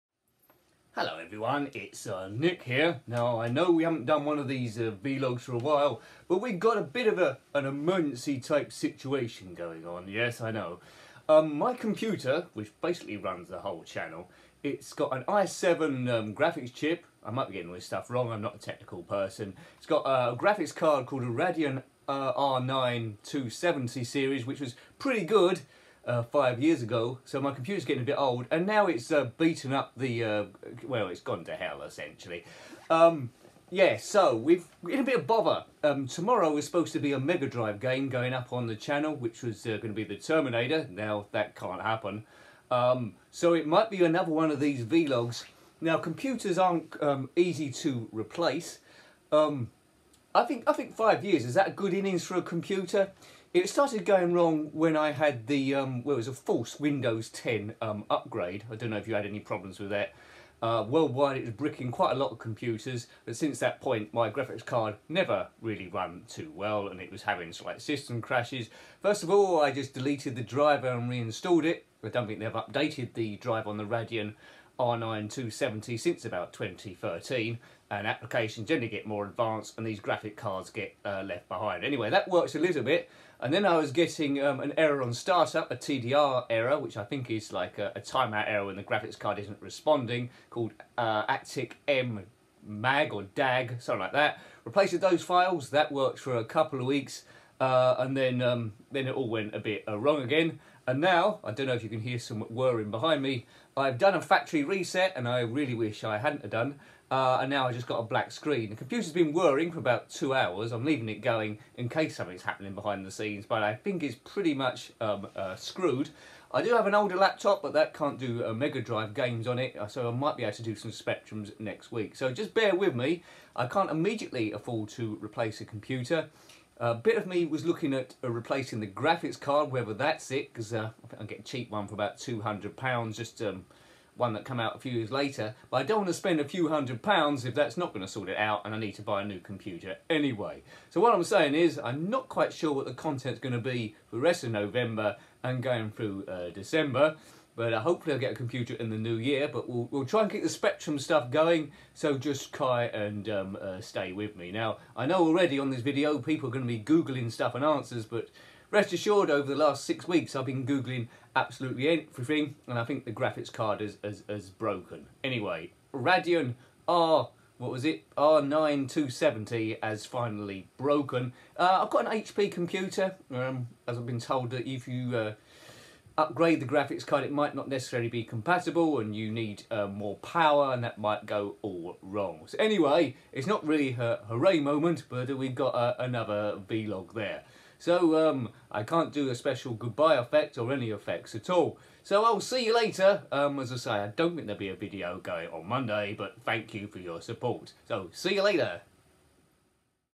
Computer Ouch - Emergency, emergency, there’s an emergency going on!
COMPUTER OUCH - Emergency, Emergency, there’s an emergency going on - It’s an announcement type thing. Have run into a spot of PC bother. Is affecting vid production somewhat. Gah!